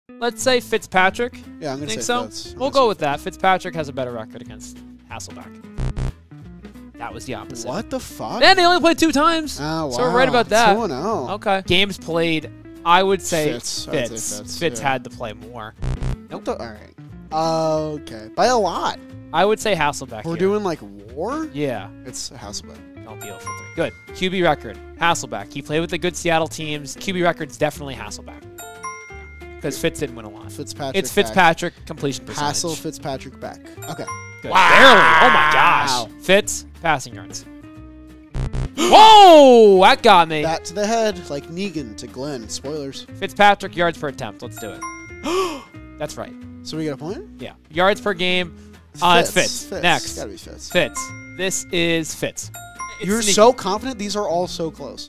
Did Ryan Fitzpatrick or Matt Hasselbeck have the better career?
Featuring: Jolly Olive and Dalton Feely
Edited by: Noah An